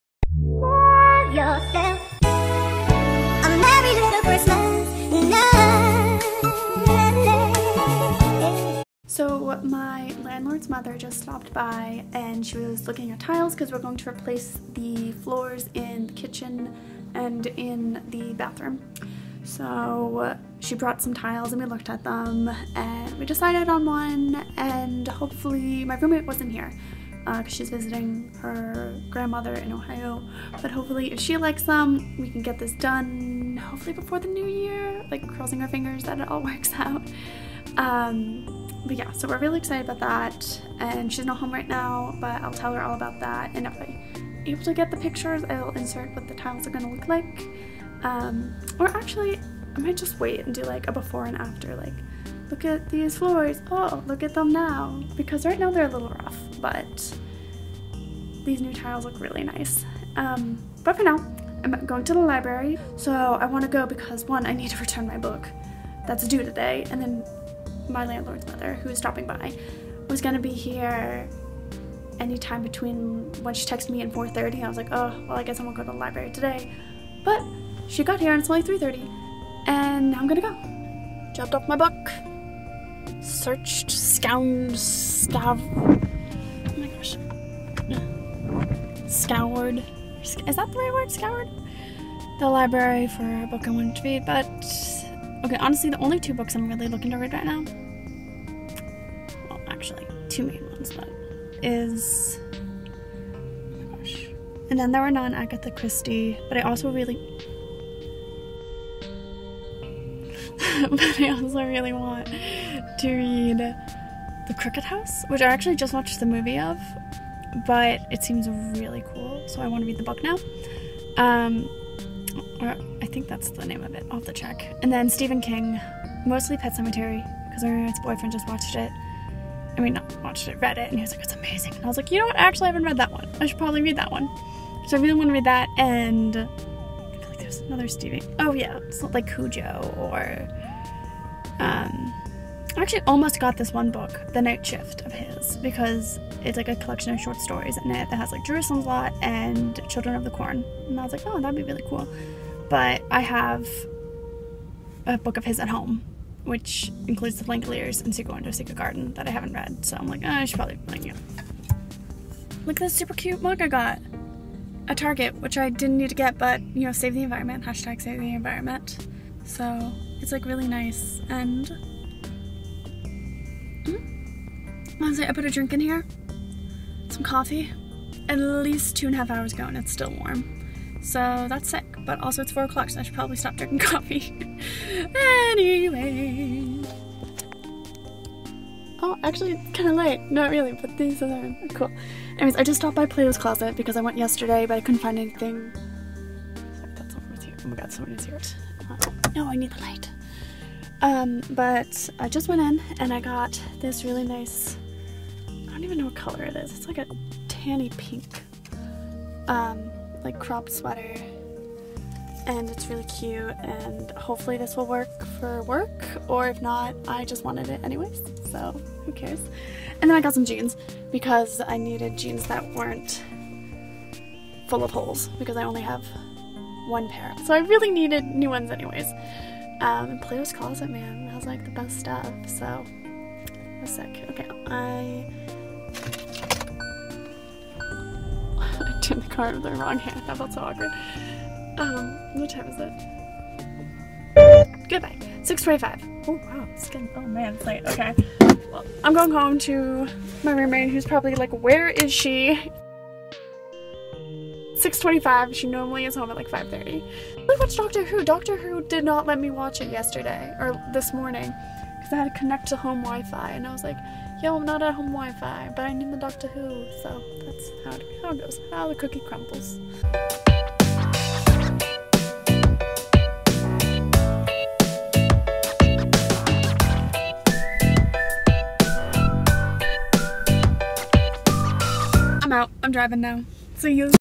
CHIT CHATS IN THE CAR | vlogmas day 10
sit down, buckle in.
I'm taking you for a spin.
as I film in my car and talk about my life and how I somehow ended up spending too much time and effort leaving the library empty handed but still bought clothes.
who am I becoming??

Peace and Blessingz,
KXK